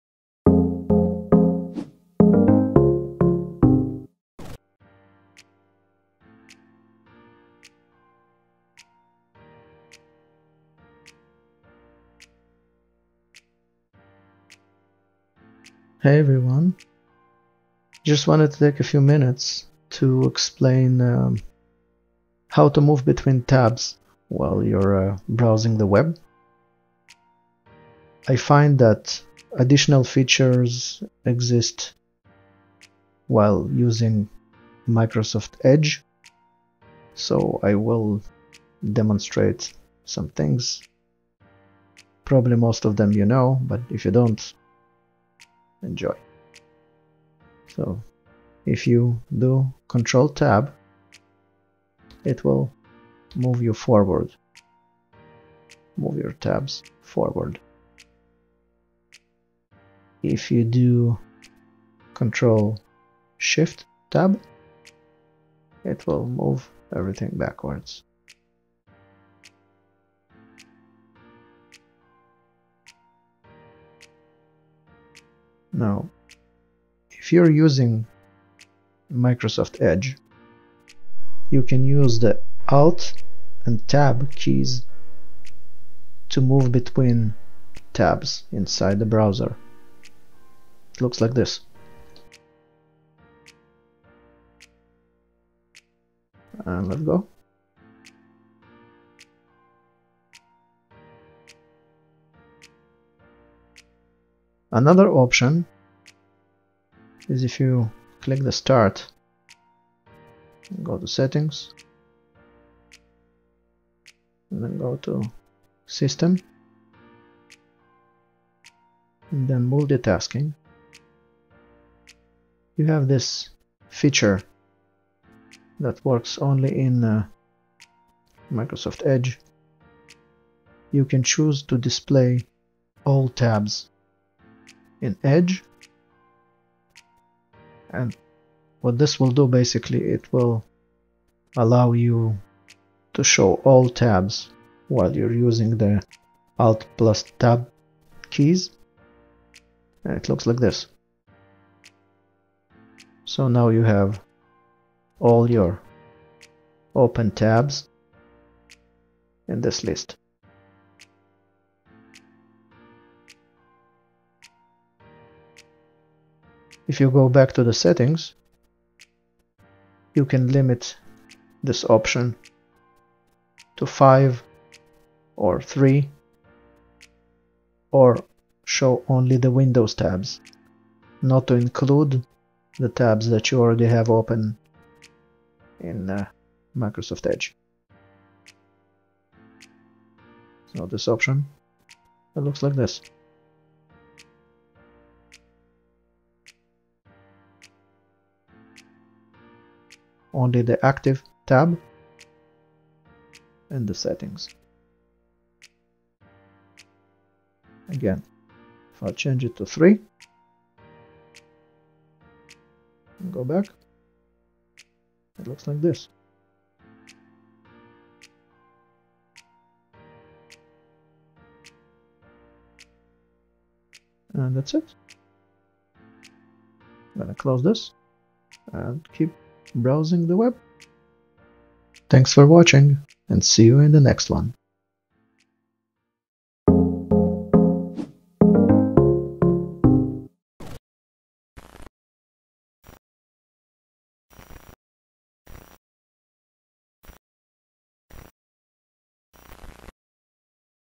Move Between Tabs in Microsoft Edge [special features]: How to do it?
Did you know you can now use Alt + Tab to move between Microsoft Edge's most recent tabs? Check out this video to learn how.